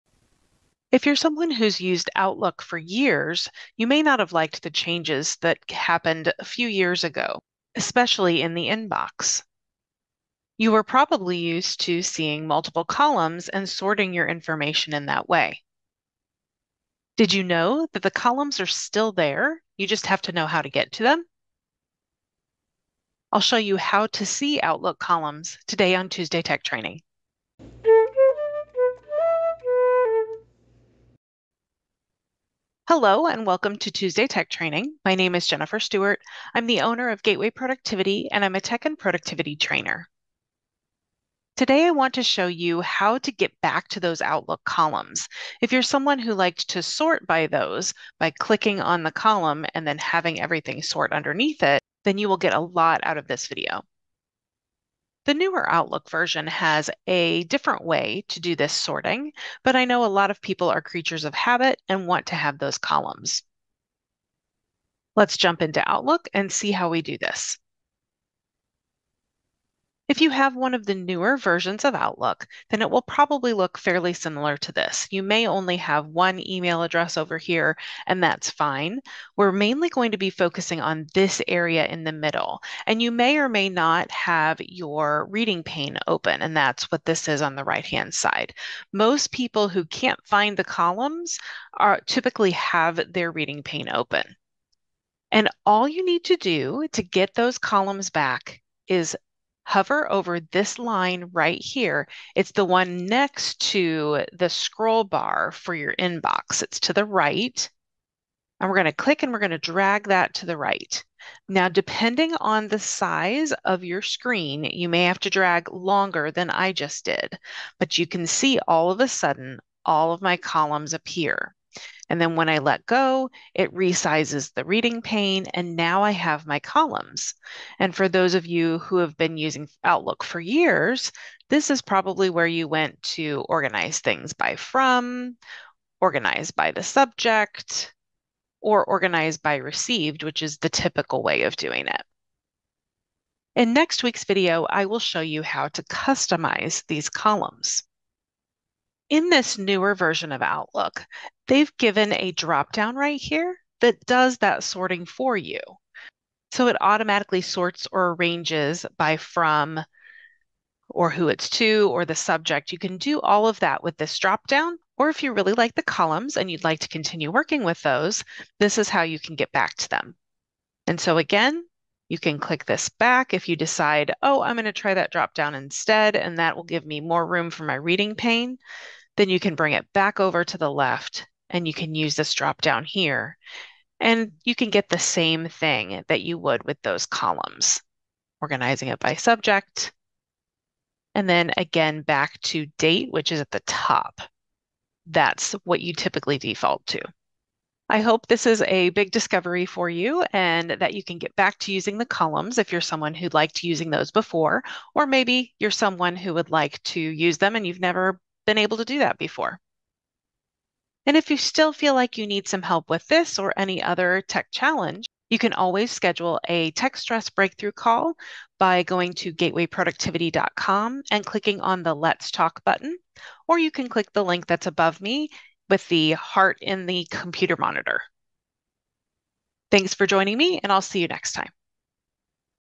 How Do I See Outlook Columns?
Tuesday Tech Training is a video series to help you better navigate your technology and increase productivity. In this video, learn how to see the columns in Outlook that were automatically there in previous versions. In next week's video, learn how to use and customize the Outlook columns.

0:00 - Introduction
1:05 - How to See Your Outlook Columns
2:10 - How to Sort Using Outlook Columns
2:32 - How to Sort Using the Dropdown Menu